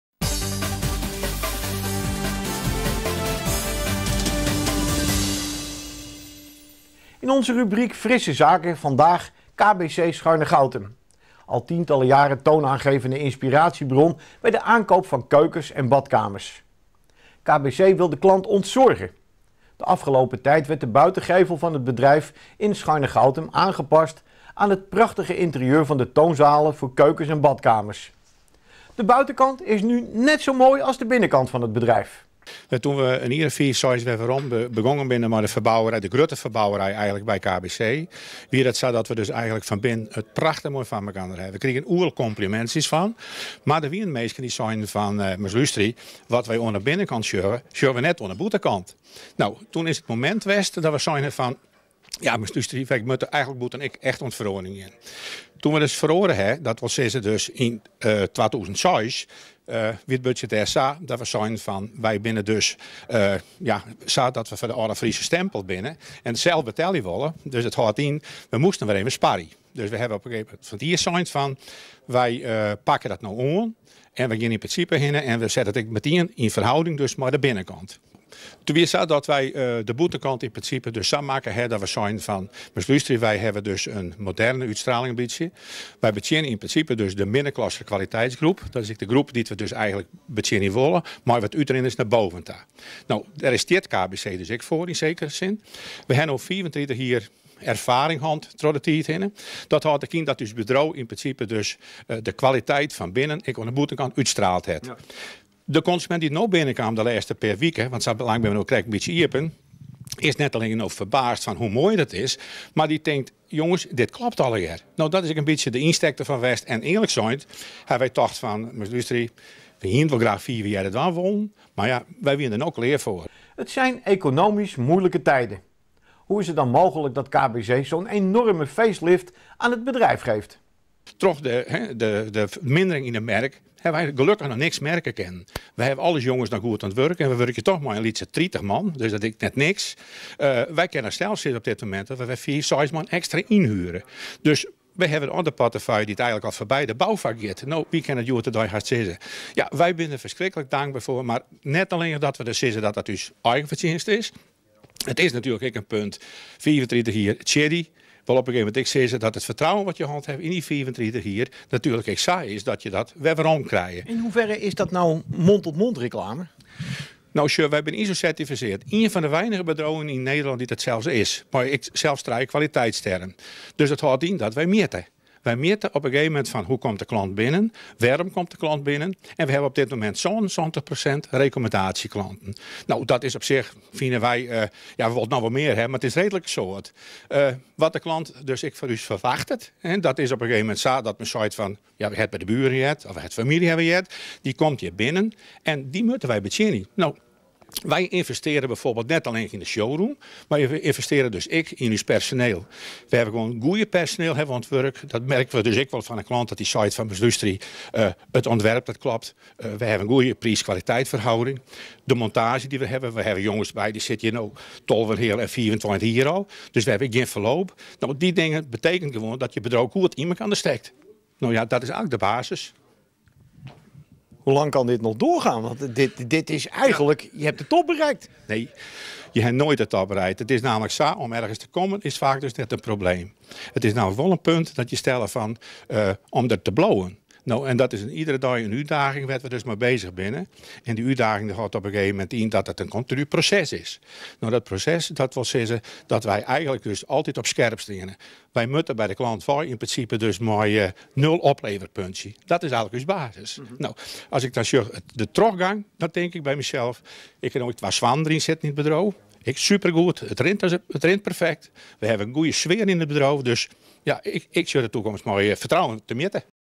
GPTV: Frisse Zaken: KBC Scharnegoutum
In onze rubiek Frisse Zaken vandaag KBC Scharnegoutum. Al tientallen jaren toonaangevende inspiratiebron bij de aankoop van keukens en badkamers. KBC wil de klant ontzorgen. De afgelopen tijd werd de buitengevel van het bedrijf in Scharnegoutum aangepast aan het prachtige interieur van de toonzalen voor keukens en badkamers. De buitenkant is nu net zo mooi als de binnenkant van het bedrijf.
Het zijn economisch moeilijke tijden. Hoe is het dan mogelijk dat KBC zo´n enorme facelift aan het bedrijf geeft.